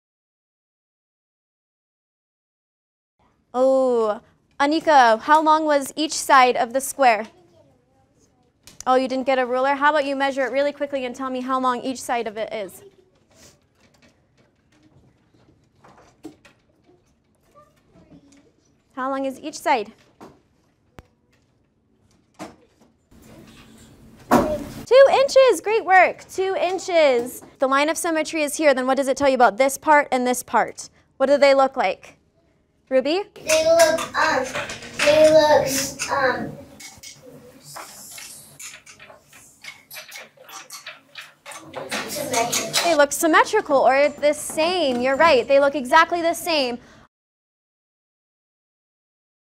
Peek Inside a Challenger Grade 1 Math Lesson | Challenger School
Take a peek inside one of our first-grade math classes, where students are learning how to measure. Notice how the teacher patiently gives her students time to think so they can, in turn, answer questions confidently and correctly.